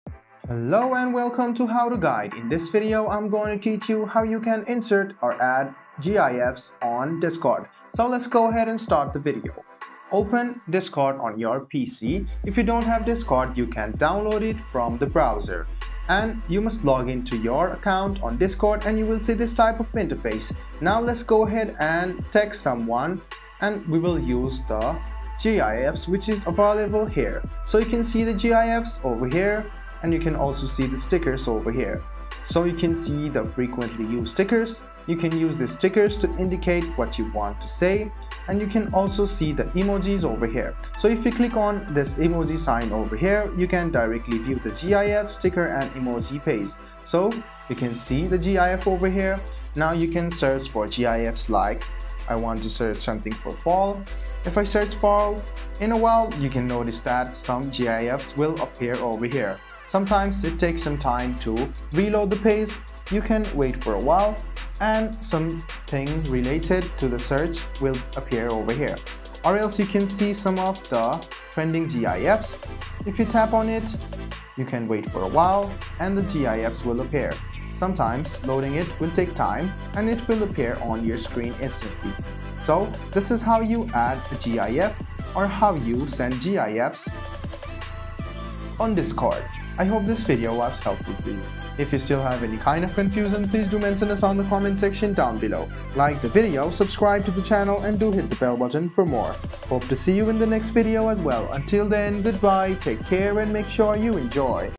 How to Add Gifs in Discord (2024)
Learn how to add GIFs in Discord with this easy step-by-step tutorial! We'll show you how to search for GIFs, add them to your messages, and make your chats more fun. Perfect for beginners who want to add some excitement to their Discord conversations.